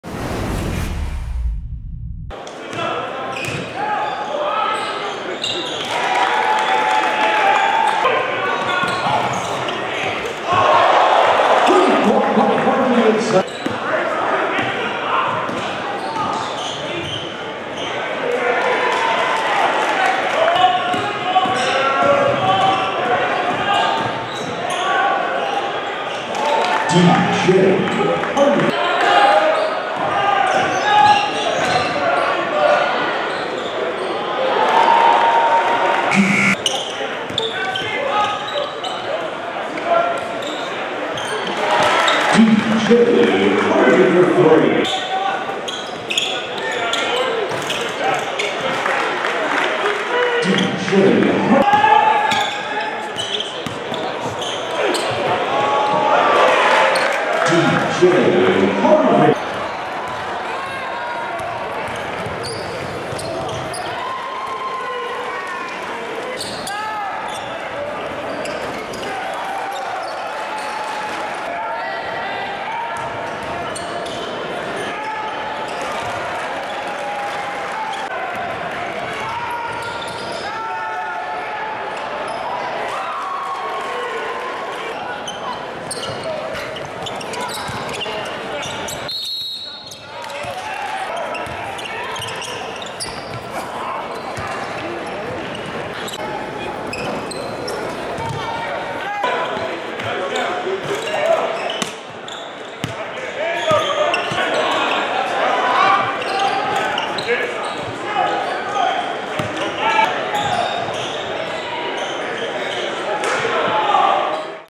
Notre Dame Commit D.J. Harvey Senior Season Highlights
Four-Star small forward DJ Harvey highlights from his senior season with DeMatha High School.